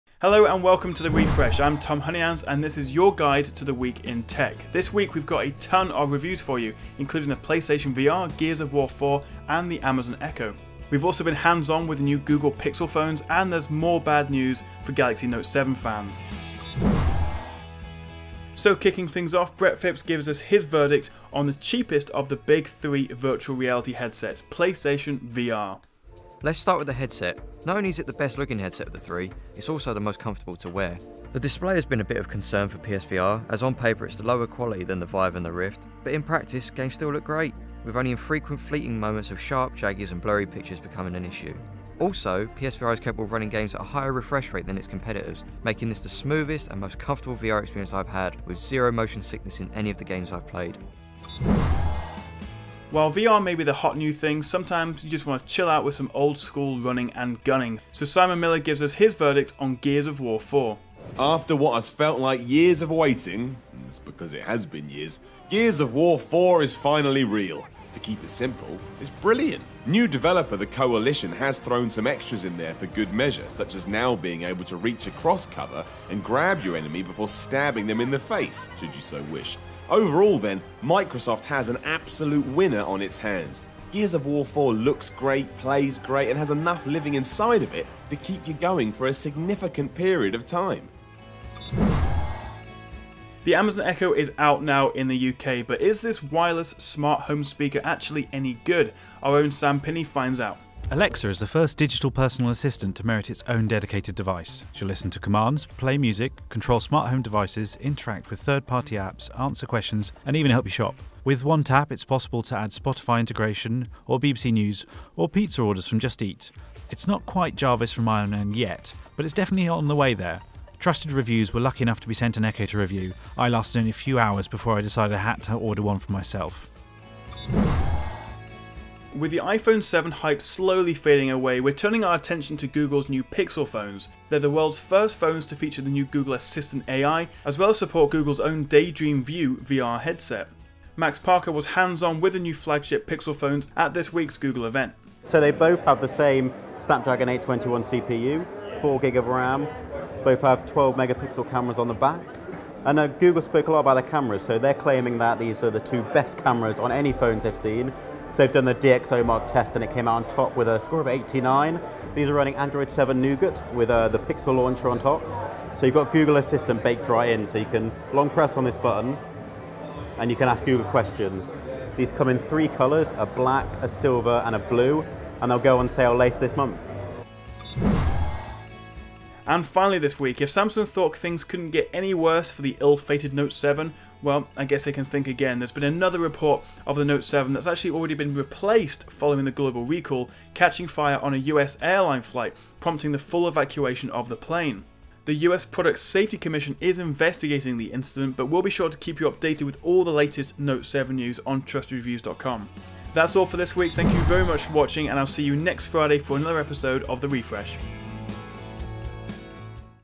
PlayStation VR is a Smash Hit, Google Goes to War with Apple | The Refresh
This week's top tech stories featuring Playstation VR review, Gears of War 4 review, Amazon Echo review, Google Pixel Hands-On and Galaxy Note 7 fire news